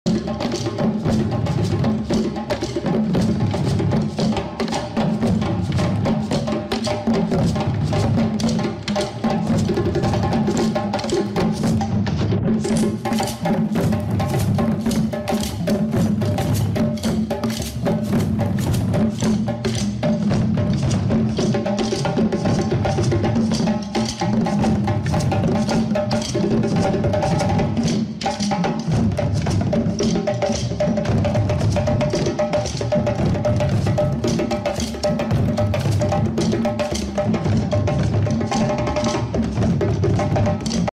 Drum Circle at the Cybrarium!
Come join us this summer for our AllTogetherNow Summer Reading Program! One recent event was a drum circle led by the amazing drummer Baba Moussa Soumare. Listen to the music that our Cybrarium patrons made and check out other programs we have planned this summer